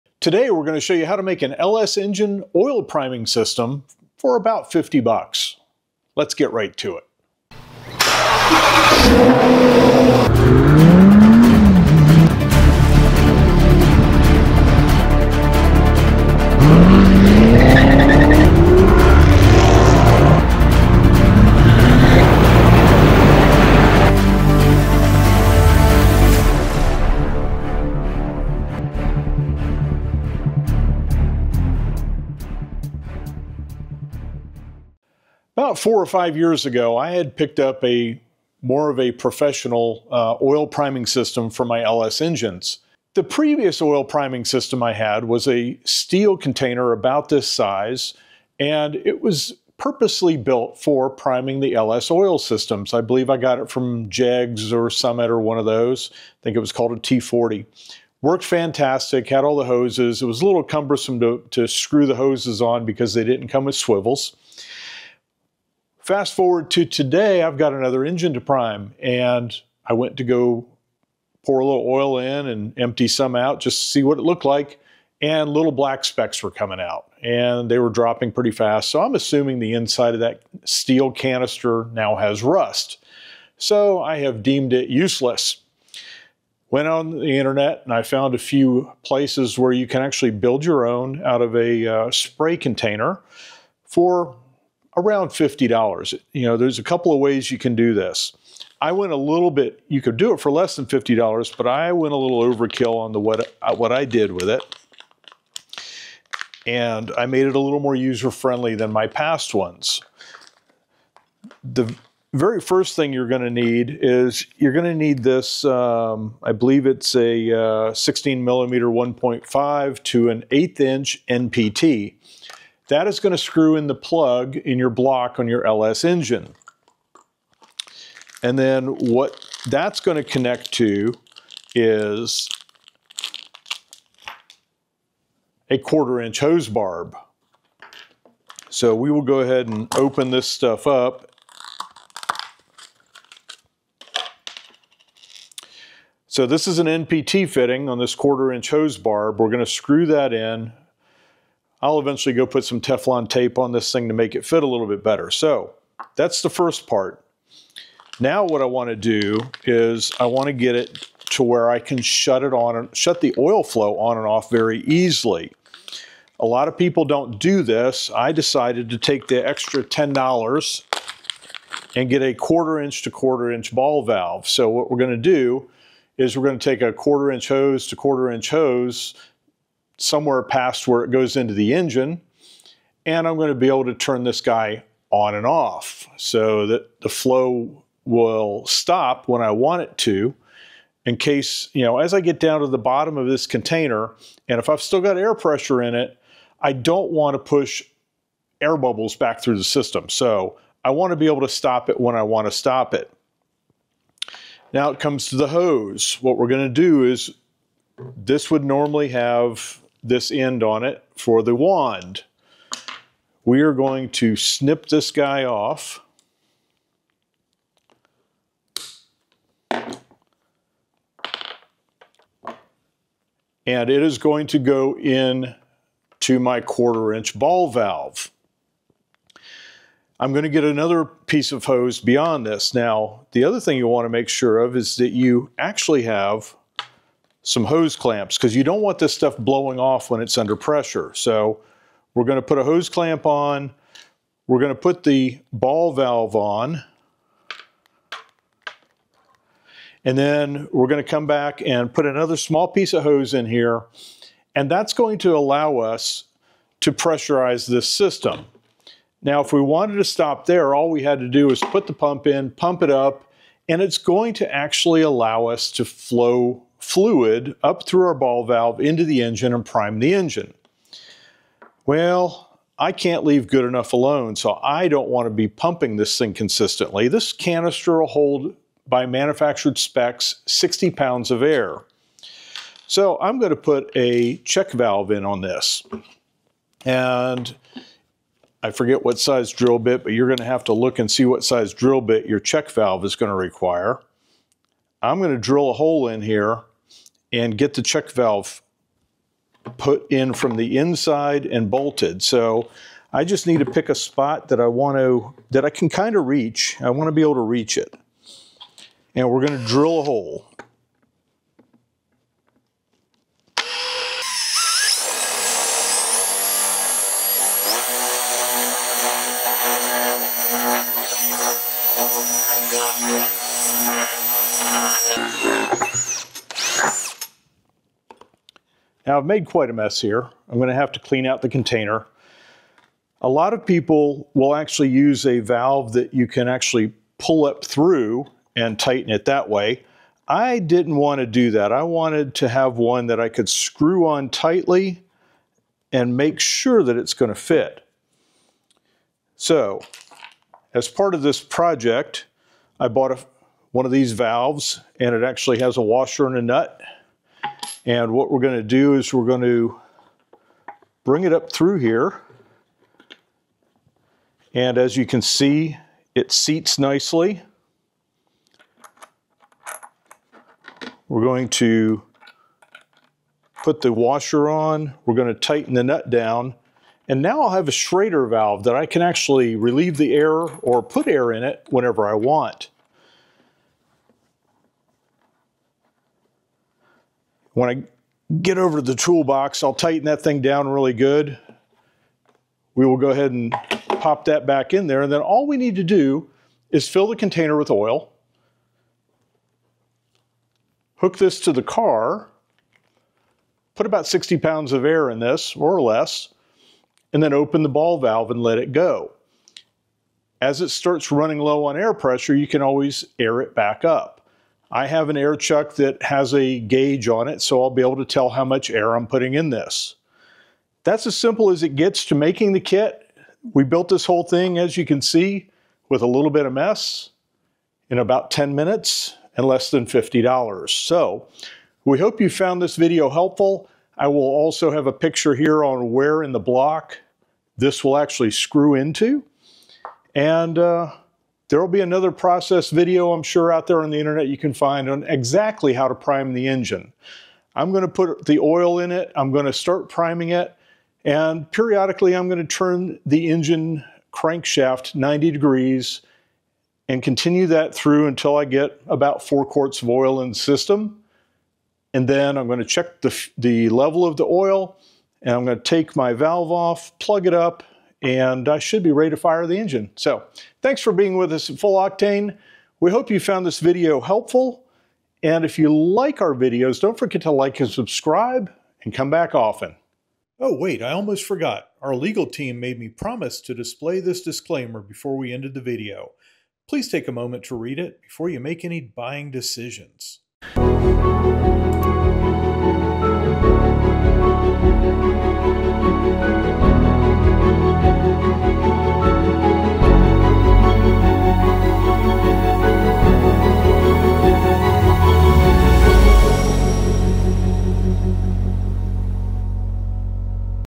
DIY LS Oil Priming Tool
This video will show you all the parts you will need and how to build a DIY engine oil priming system for a GM LS engine.

Here are the parts you will need:

Chapin 20004 Made in USA 1 -Gallon Lawn and Garden Pump Pressured Sprayer

TR618A Tire Valve Stems,Tubeless Air Liquid Tractor Valve Stems for Tubeless Tires

1/4 inch brass mini shut off ball barb valve

Stainless Steel Oil Pressure Sensor Adapter – M16-1.5 Male to 1/8" NPT Female

Insurance available in all 50 states, let us know if you would like a quote.